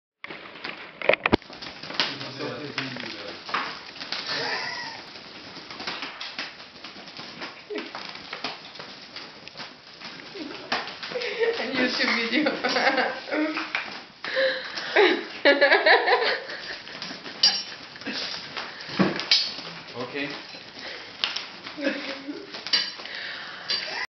Dogukan Manco playing with Dumbo (behind the scenes TAXI DRIVER, Shortfilm)
dogkan manco taxi driver behind the scenes austrian independent production vienna 2012 short film